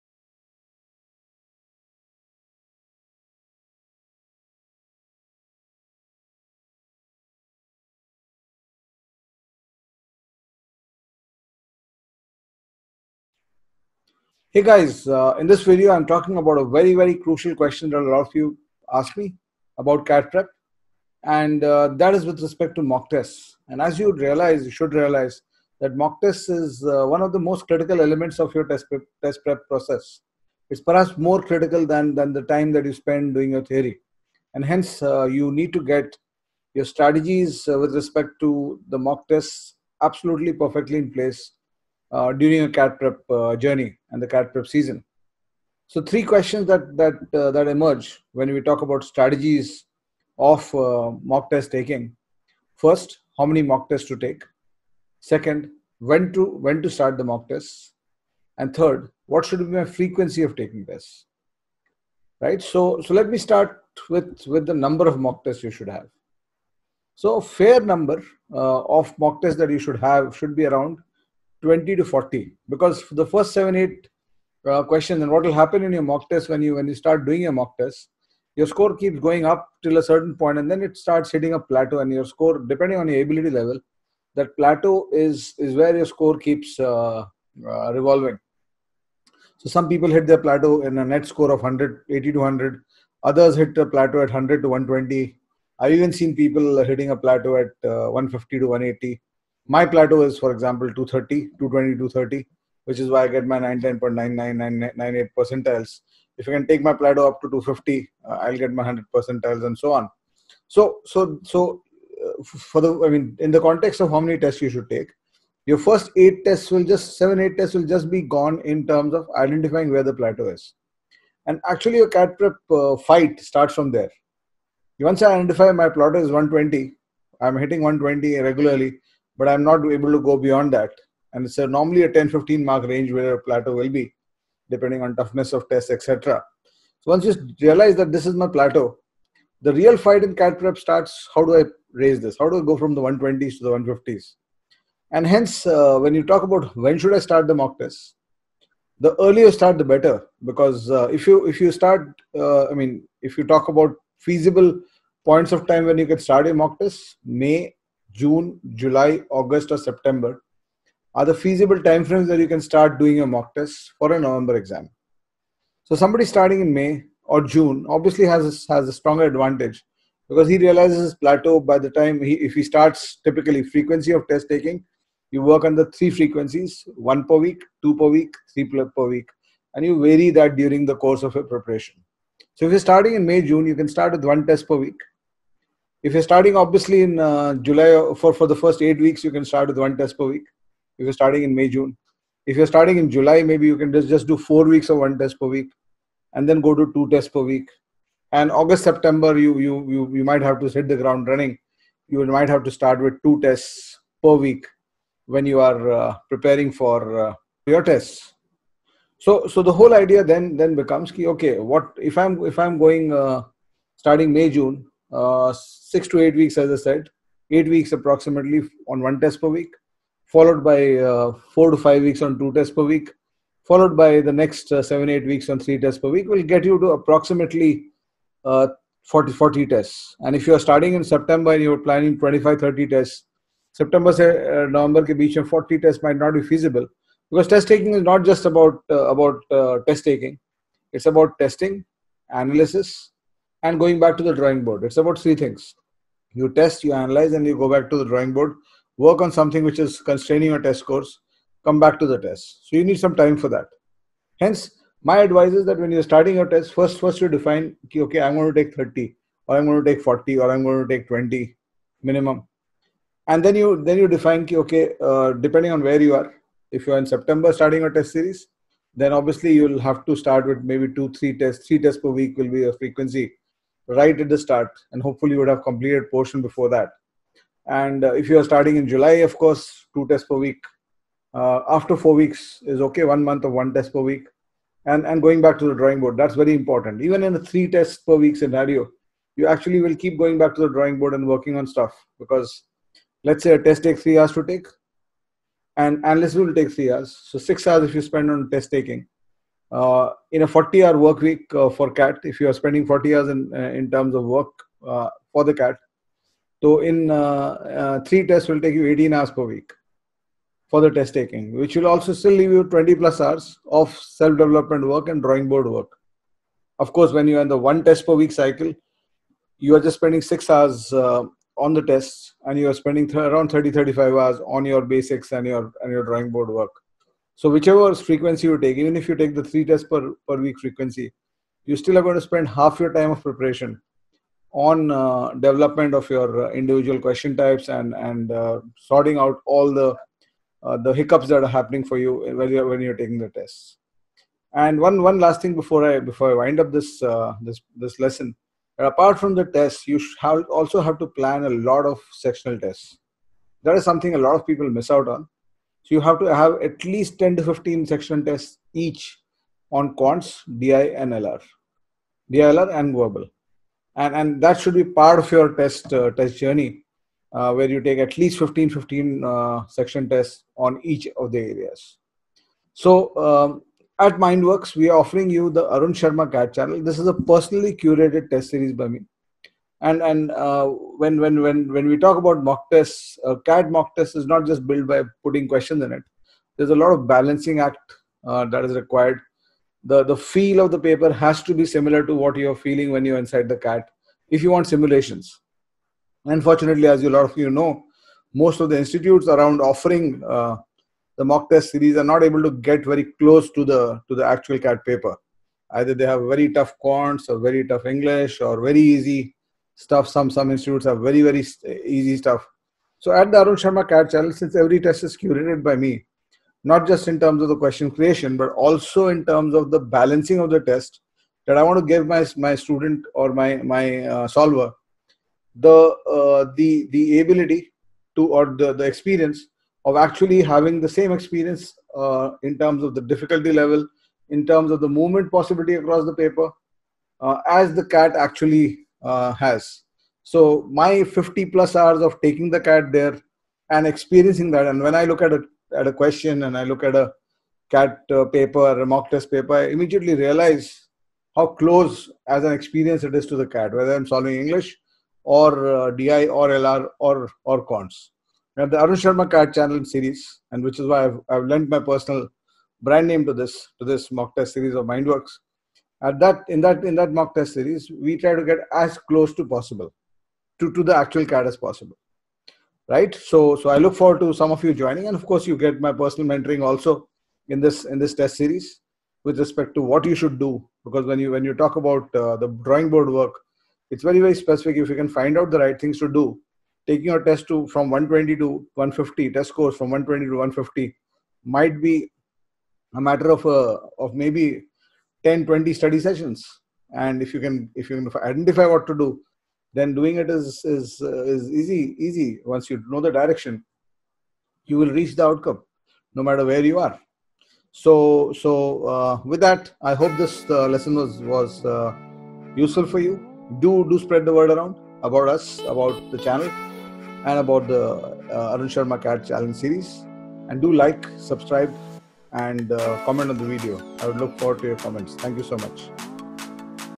How many mocks required for 99 percentile in CAT 2020
How many mock tests to take for CAT? When to start your mock tests? At what frequency to take your CAT mock tests? Arun Sharma tells you all you need to know about these questions and more around your mock test strategies for CAT.